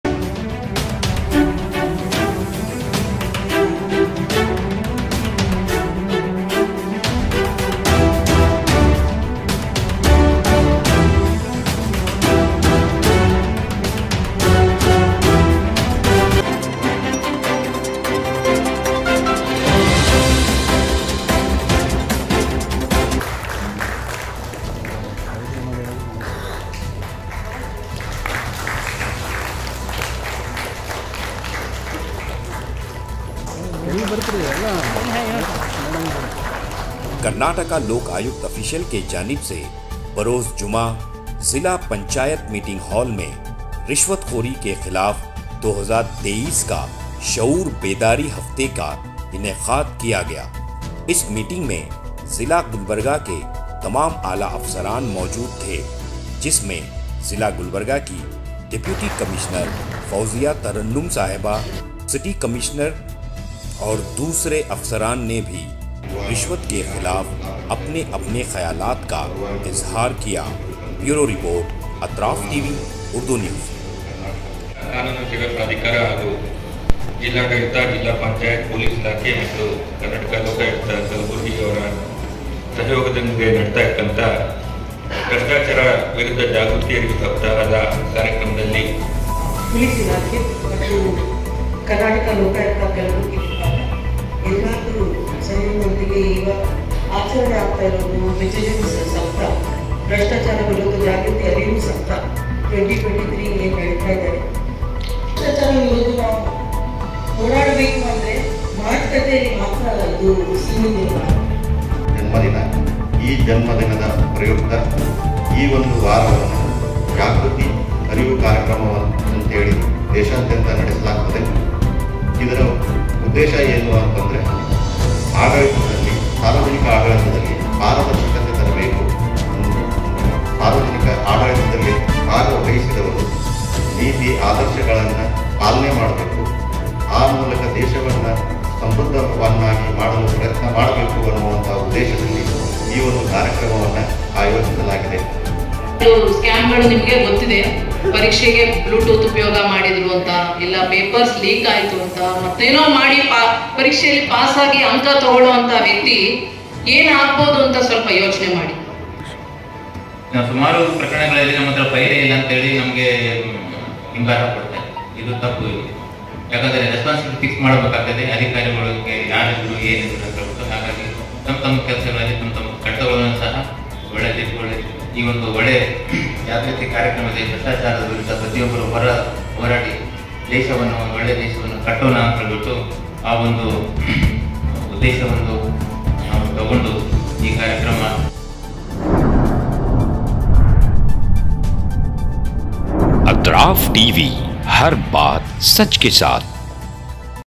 KARNATAKA LOKAYUKTA ZILA PANCHAYAT MEETING HALL MEIN RISHWAT KHORI KE KHILAF SHAUR BEDARI KA INEQAD
KARNATAKA LOKAYUKTA OFFICIAL KI JANIB SE BAROZ JUMA ZILA PANCHAYAT MEETING HALL MEIN RISHWAT KHORI KE KHILAF 2023 KA SHAUR BEDARI HAFTE  KA INEQAAD KIYA GAYA IS MEETING MEIN ZILA GULBARGA KE TAMAAM ALA AFSARAAN MAUJOOD THEY JIS MEIN ZILA GULBARGA KI DEPUTY COMMISSIONER FAUZIYA TARRANNUM SAHEBA AUR CITY COMMISSIONER NE RISHWAT KE KHILAF APNE APNE KHAYALAAT KA IZHAAR KIYA AUR DOOSRE AFSARAAN NE BHI APNE APNE KHAYALAAT KA IZHAAR KIYA |
 ATRAAF TV |